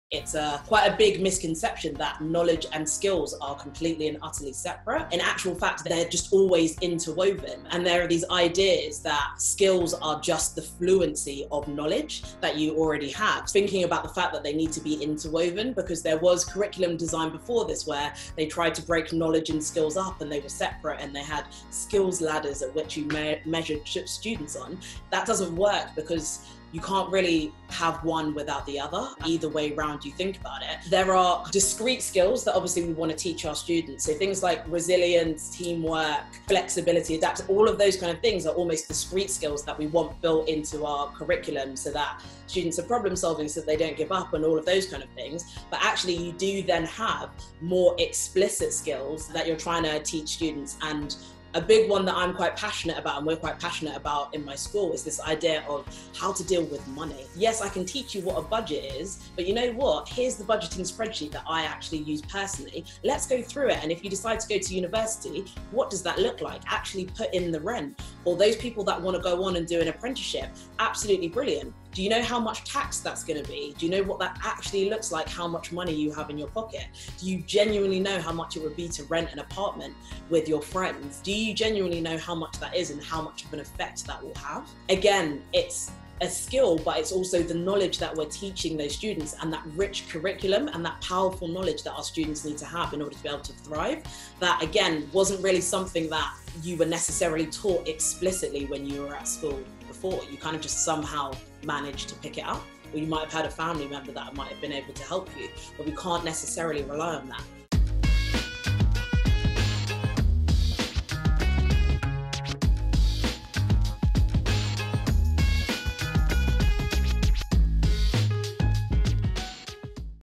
2 minutes with Natalie Dormer: Teaching important knowledge and skills in schools | Ivy House
Natalie Dormer, Assistant Principal at Harris Academy Greenwich, shares her thoughts.